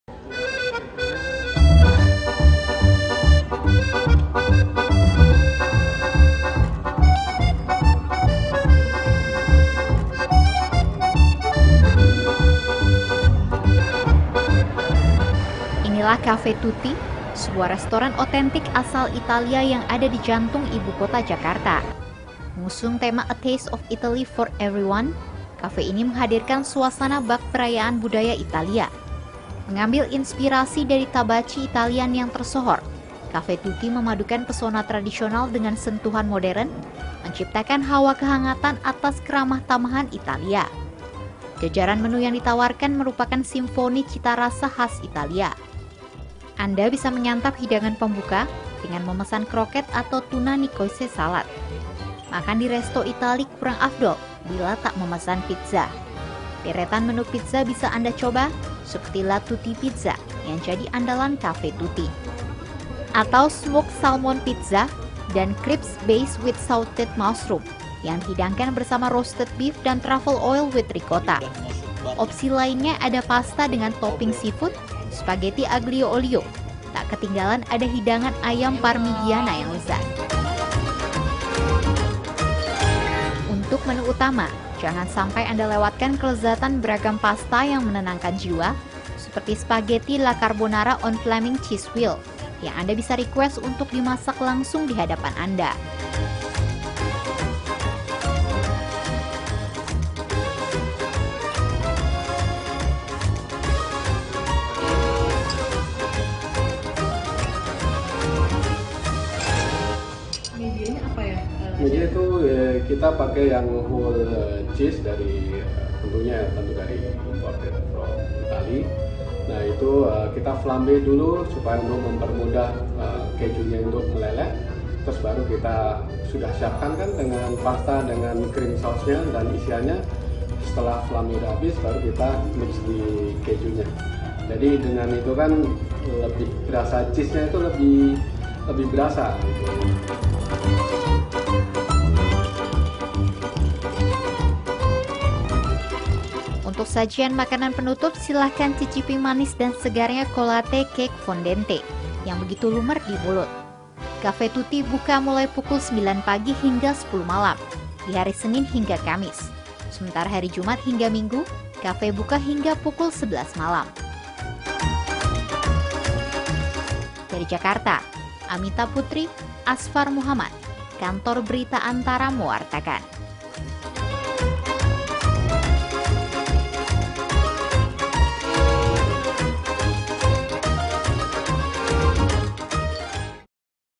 Gurihnya spageti keju creamy khas Italia di Cafe Tutti
ANTARA - Anda pecinta makanan khas Italia? Jika ya, maka Anda wajib datang ke Café Tutti yang ada di SCBD, Jakarta Selatan. Beragam menu khas Italia bisa anda jumpai di sini, mulai dari pizza, pasta, hingga hidangan penutup yang bisa memanjakan lidah. (Amita Putri Caesaria/Azhfar Muhammad Robbani/Agha Yuninda Maulana/Nanien Yuniar/Puji Lestari)